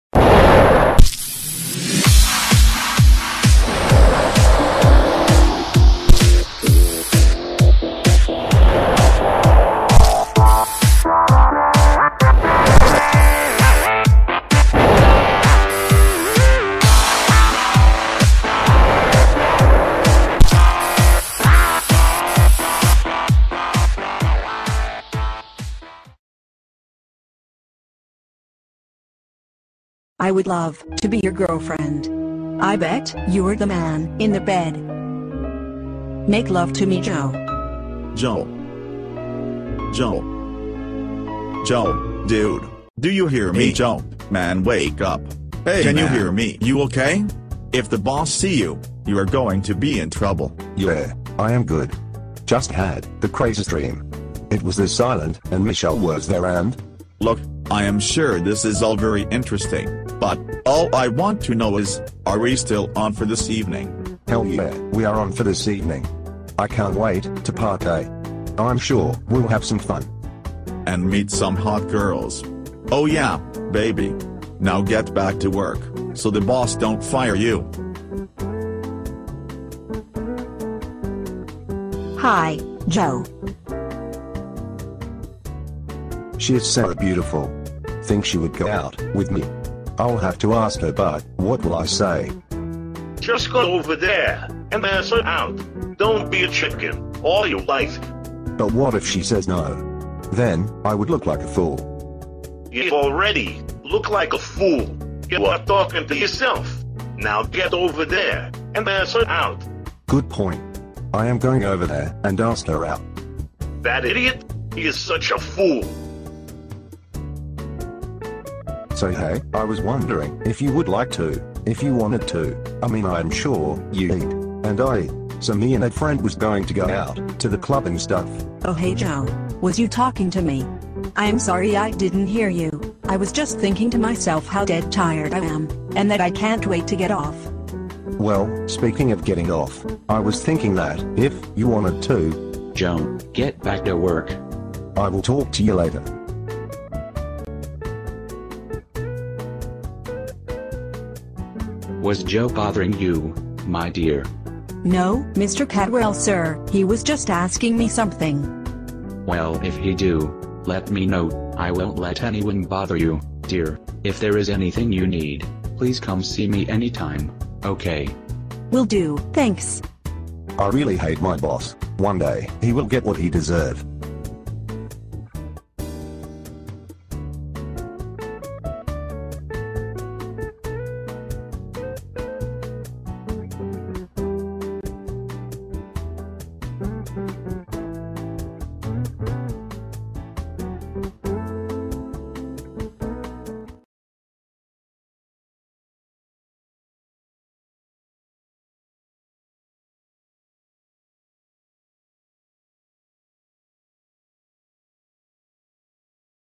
The Average Joe Episode 1
The episode where Joe and his friend plan to go out but Joe wants to ask Michele out also.

Voice is a little off because it is computer generated.  Looking for voice over actors for parts.